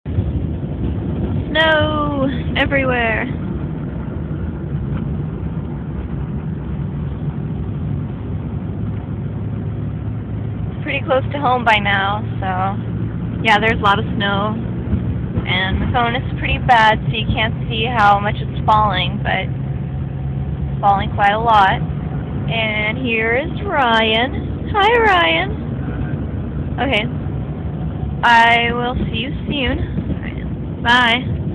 Snowy drive
The night before this video, we got stuck in a snowbank and had to spend the night in a shitty motel after getting rescued by the WA-DOT, so I made this video on my phone for my boyfriend's sister to show her we were still alive the next day.